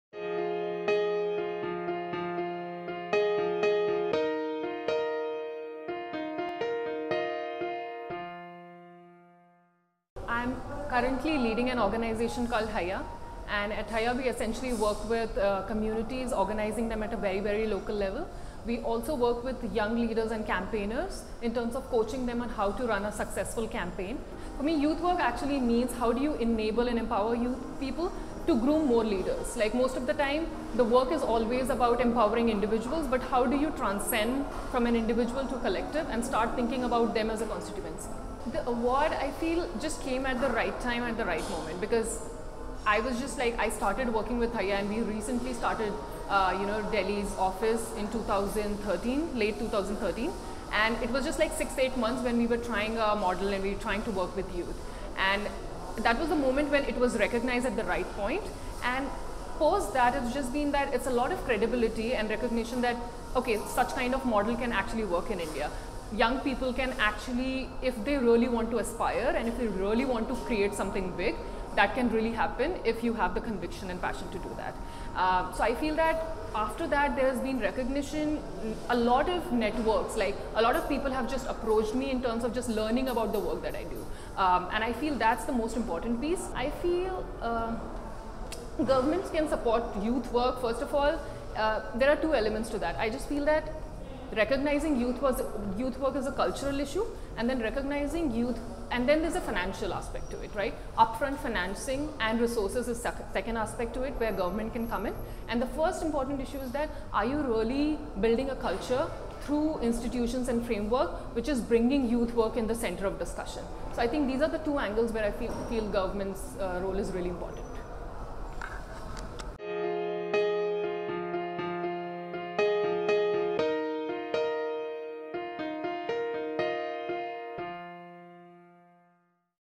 Why do we need youth workers?
Aprajita Pandey, 29, from India, is the Delhi Manager of Haiyya, a grassroots NGO formed to strengthen participatory democracy in urban neighbourhoods.
Her contribution and dedication to young people was recognised in the Commonwealth Youth Worker Awards 2014, when she was announced as the Commonwealth Asia Region Youth Worker of the Year.